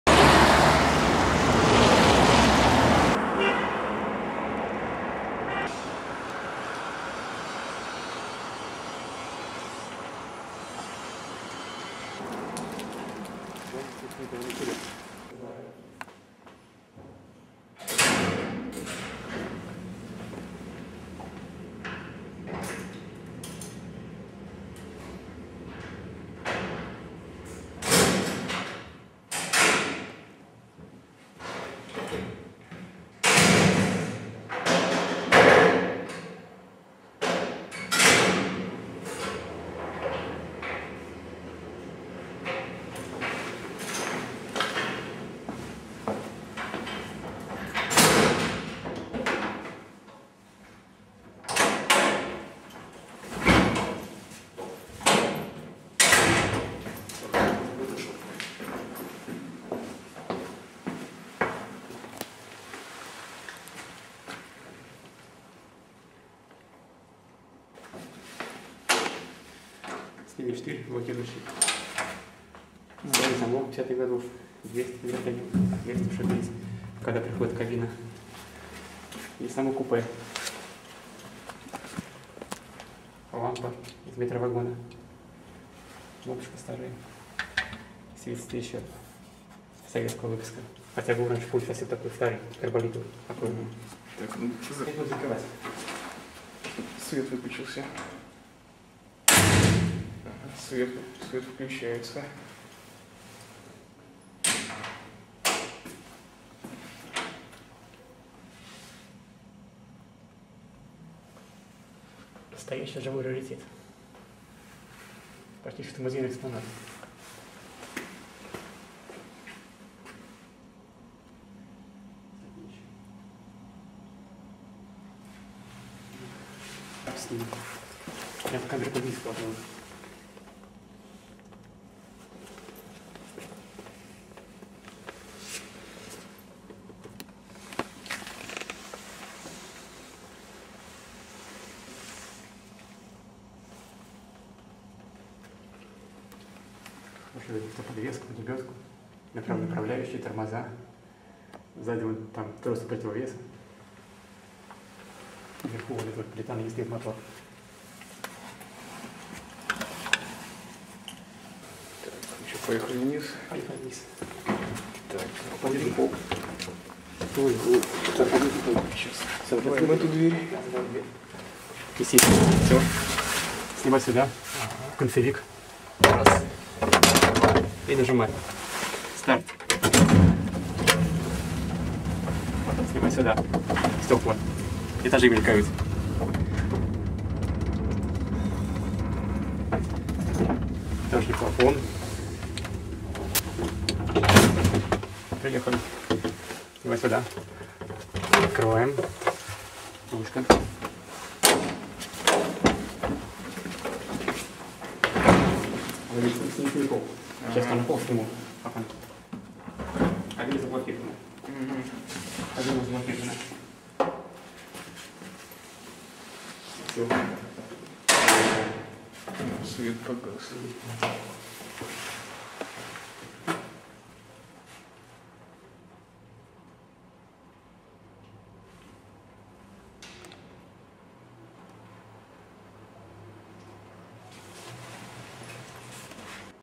Лифт с распашной дверью, Москва, Верхняя Масловка 23. 2012-й год.
Верхняя Масловка. Лифт с ручной распашной дверью. Виды дверей, интерьер кабины, сьёмка в движении снаружи, работа дверями.
Moscow, old elevator.